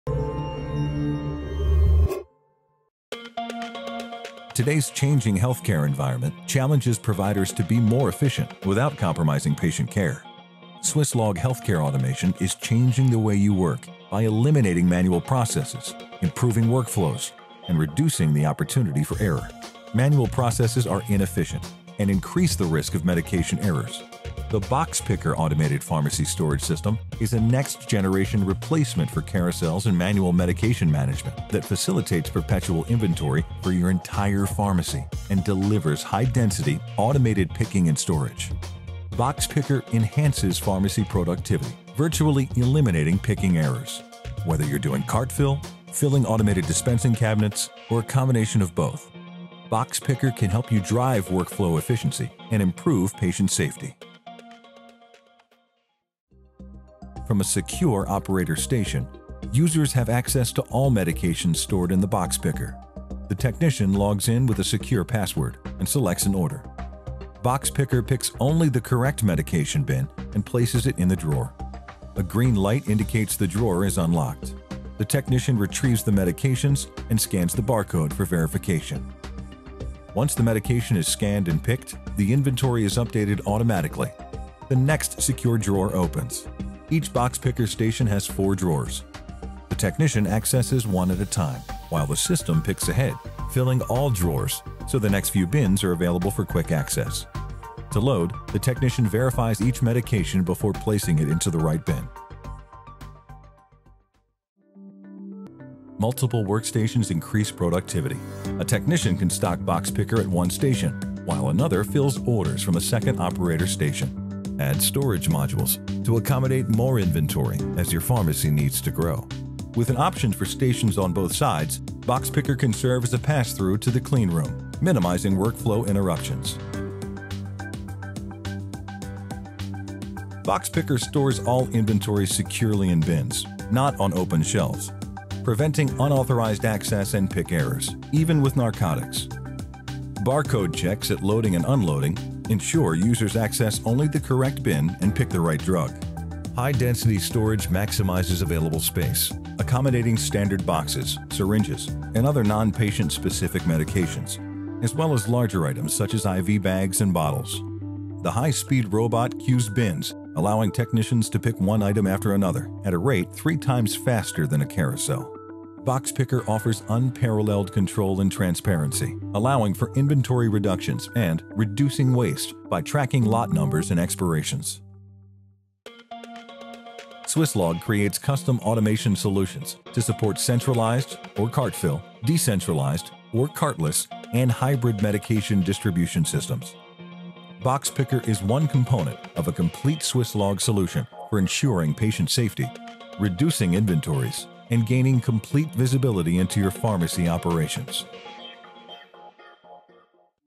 BoxPicker Automated Pharmacy Storage System
The BoxPicker Automated Pharmacy Storage System provides secure, high-density storage and dispensing of medications and supplies. This high-performance system controls access, improves pharmacy workflow efficiency and expedites the picking process. A robotic replacement for vertical carousels, refrigerated drug storage and narcotic vaults, BoxPicker fully automates medication storage and retrieval steps to eliminate picking errors and increases safety at dispense.